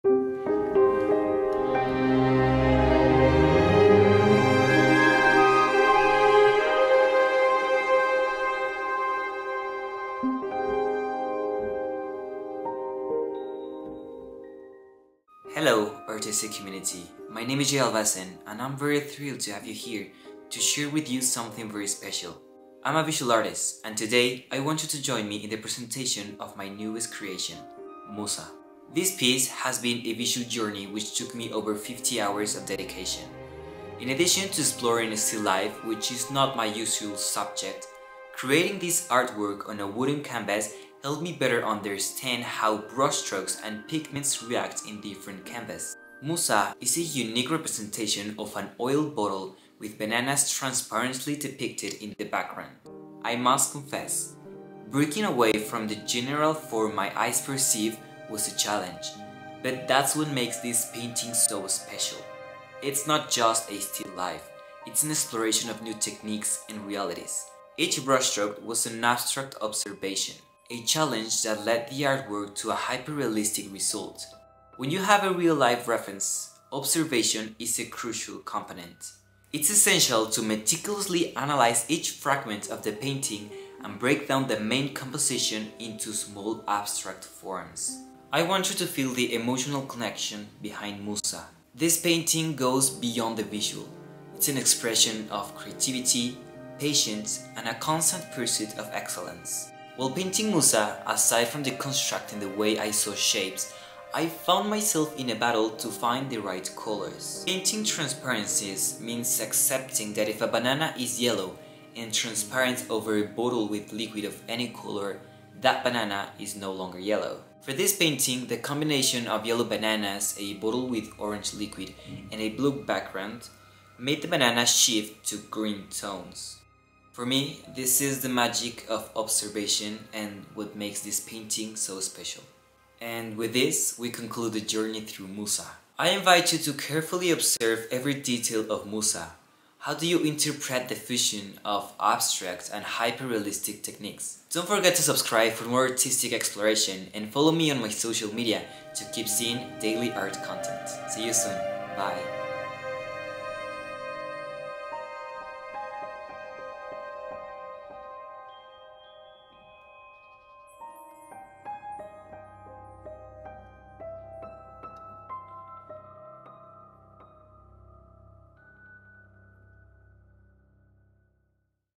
Hyperrealistic Painting: Musa by Jay Alvasan
Hello, art community! I'm Jay Alvasan, and I'm thrilled to share my latest artwork, "MUSA." This 50-hour visual exploration on wood takes a departure from my usual style.

Join me in this video to discover the challenges and techniques behind "MUSA," a unique representation of an oil bottle with transparent bananas, defying norms and highlighting the art of observation.

Explore the emotional depth of "MUSA," an expression of creativity and patience. Decode the fusion of abstract and hyperrealistic techniques and unravel the subtle shades in seemingly yellow bananas.